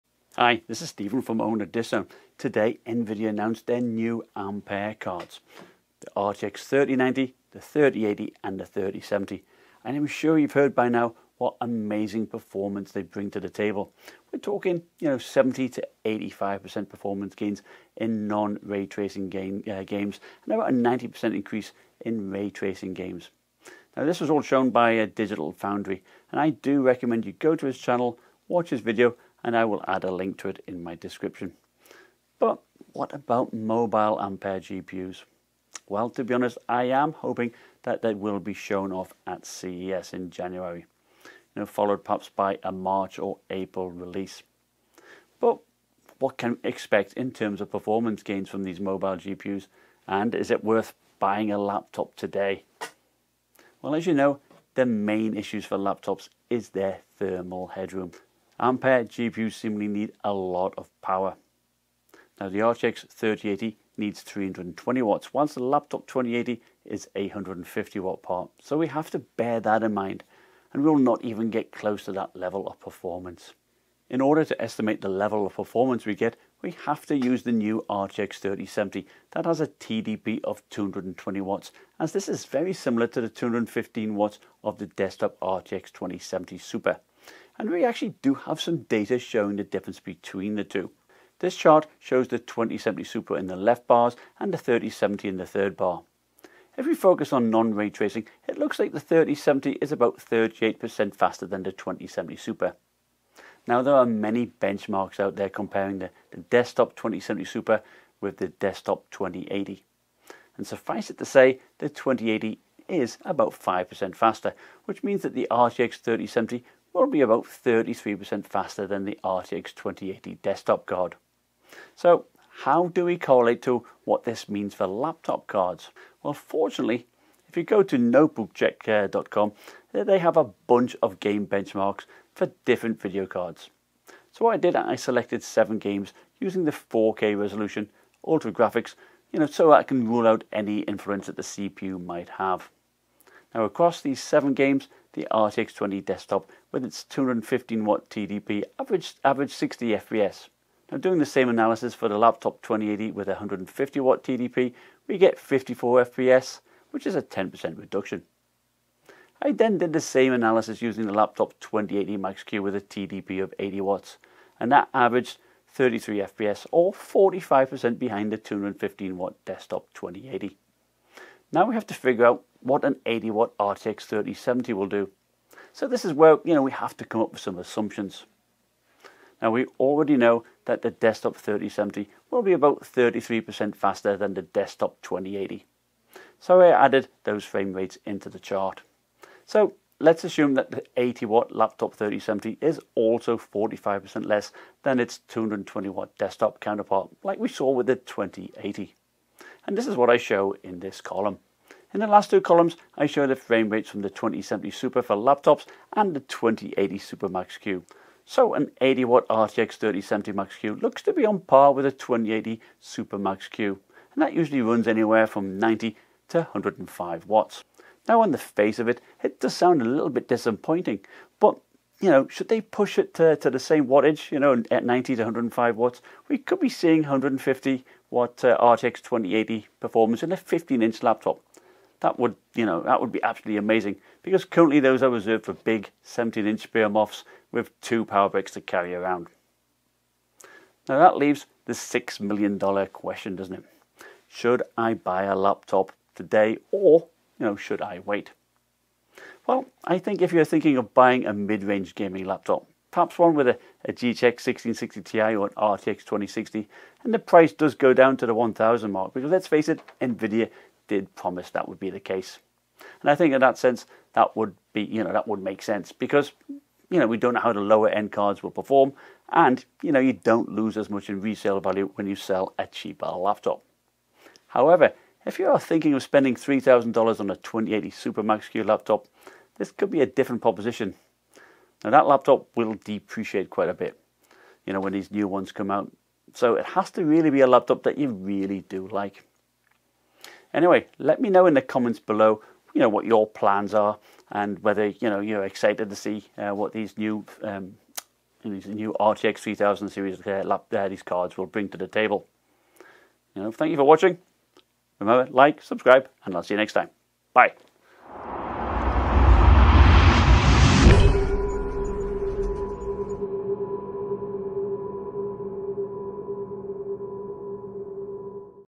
RTX 3070/RTX 3080 For Laptops - How Fast & Should I Wait ?
Get high-quality skins & wraps for your laptops, phones, and handhelds here 👉 m2skins.com Today, Nvidia announced their new RTX Ampere line up: RTX 3090, RTX 3080 and the RTX 3070 and it is without doubt the best performance jump we have seen. However, with the power limitations and thermal headroom that exists for laptops we cannot expect the same level of performance. So what can we expect and should we buy a laptop or wait till the new mobile RTX Ampere laptops are released?